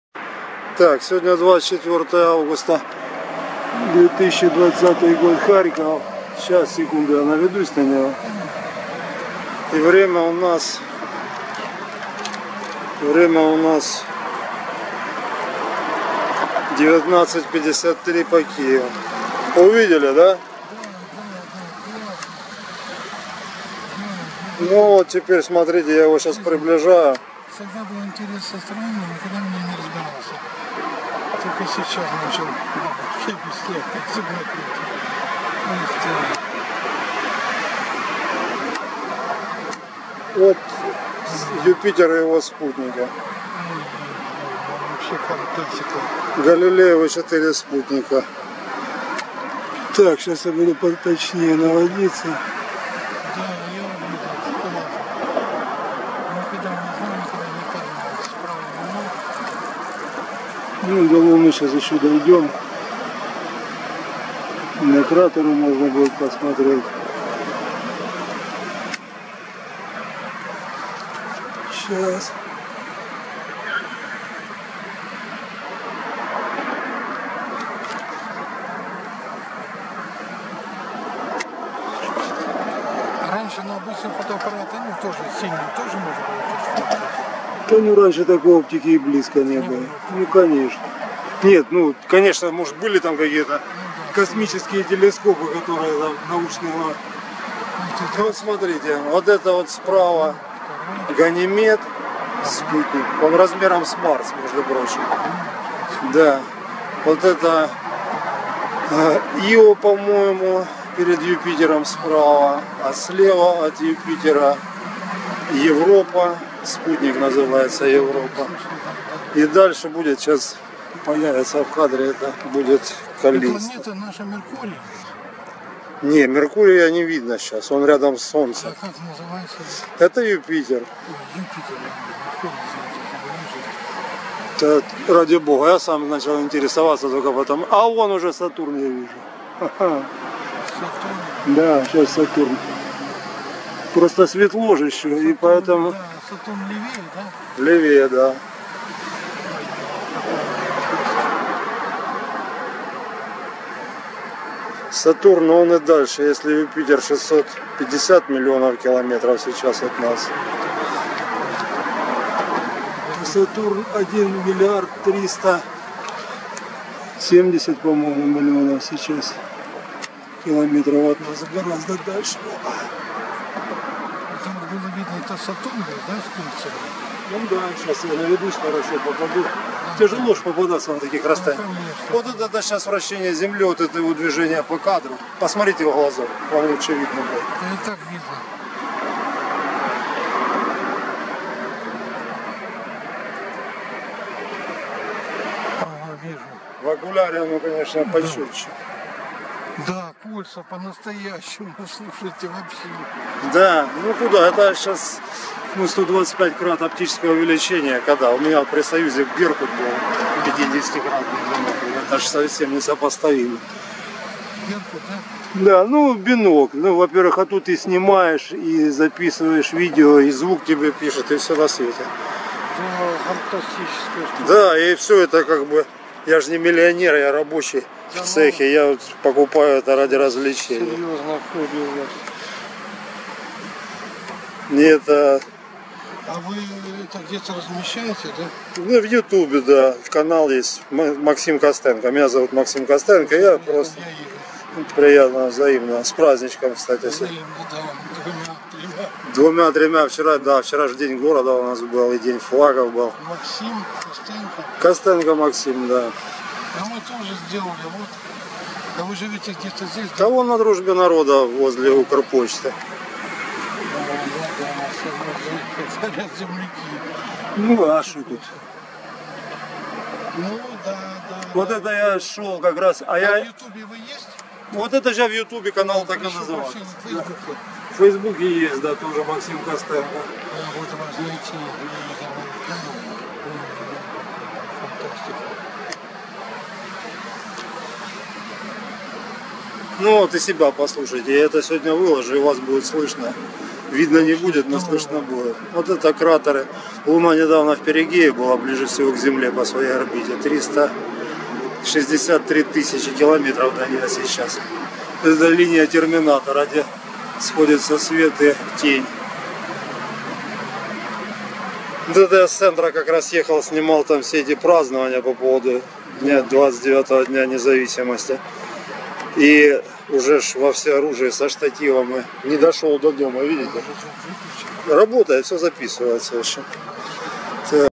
Юпитер и спутники, Сатурн с кольцами и кратеры Луны над Харьковом 24 августа 2020 года
В сумеречном небе вечером 24 августа над Харьковом встаёт Юпитер с галилеевыми спутниками и Сатурн с кольцами - серп растущей Луны уже клонится к горизонту.